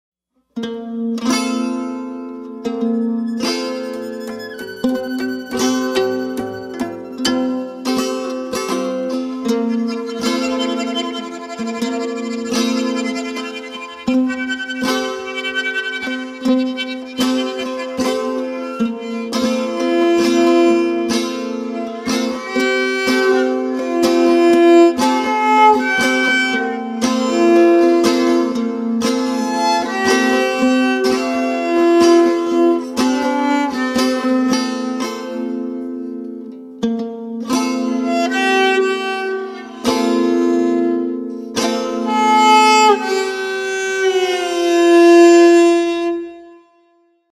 Violele quick improv during final setup
a moment of beauty found during final setup of my customer’s violele